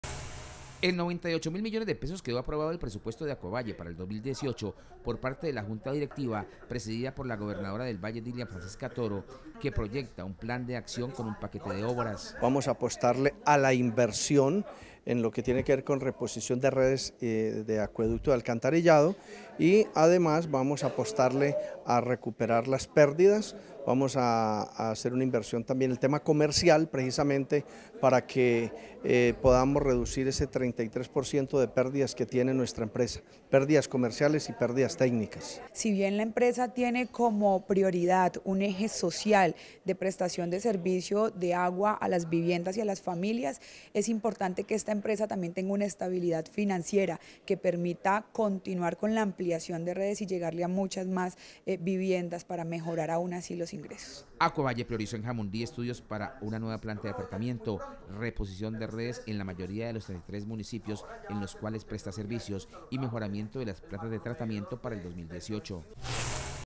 Aprobado en $98 mil millones el presupuesto de Acuavalle para el 2018
Emitido el 28 de Diciembre de 2017 a la 1:30 p.m. por el Canal regional Telepacífico.